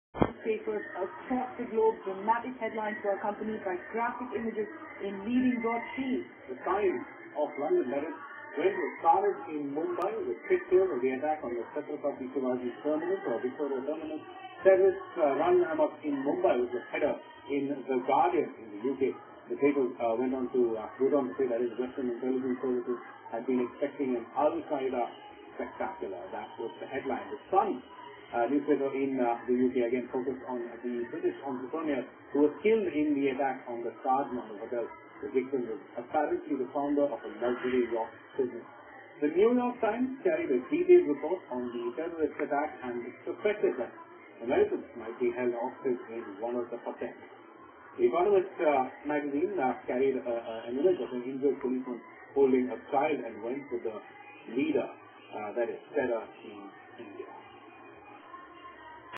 Mumbai Terror Attacks (Worlds Newspapers).wmv
Worlds newspapers reactions to Mumbai's Terror Attacks.
Courtesy: CNN IBN